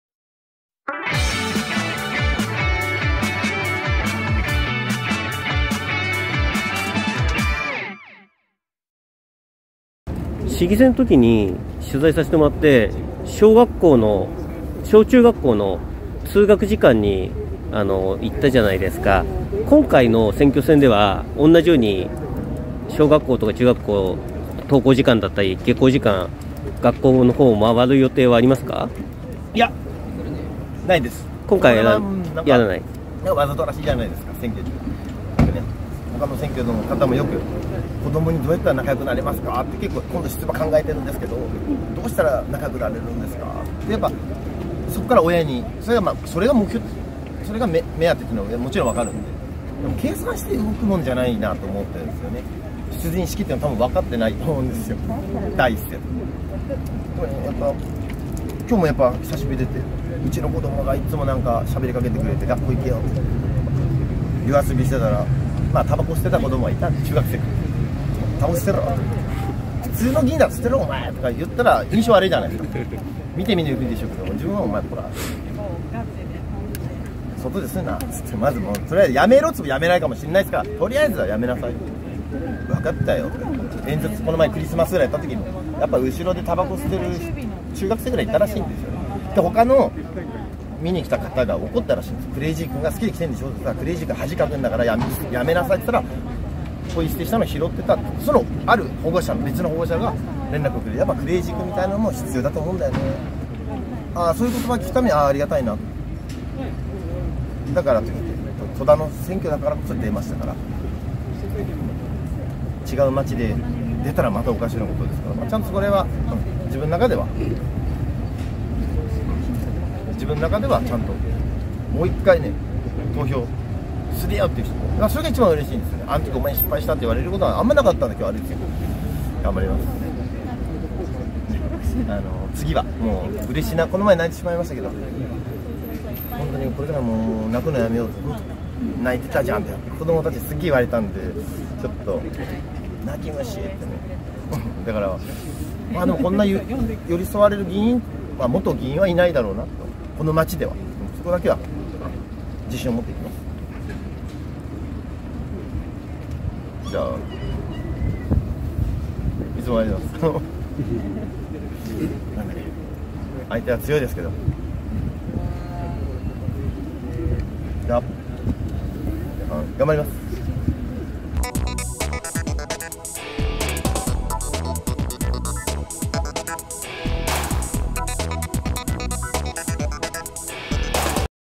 スーパークレイジー君 戸田市長選挙 第一声後 ぶら下がり会見 小中学校の登下校時間に学校へ行く？
スーパークレイジー君　戸田市長選挙　第一声後　ぶら下がり会見
小中学校の登下校時間に学校へ行く？